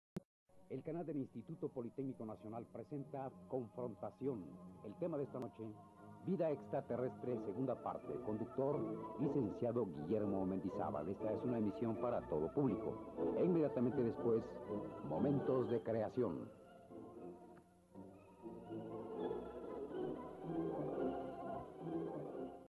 Cortinilla de Canal 11 XEIPN del Verano de 1986
Se necesitan encontrar más cortinillas de Canal 11 de la época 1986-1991.
Porfavor no sean idiotas y no me culpen por el volumen bajo del video
Cortnilla de Canal 11 grabada durante el Verano de 1986 cuando el Canal 11 apenas estrenaba cambio de imagen, ya que los micofonos del canal aún seguían teniendo el logotipo anterior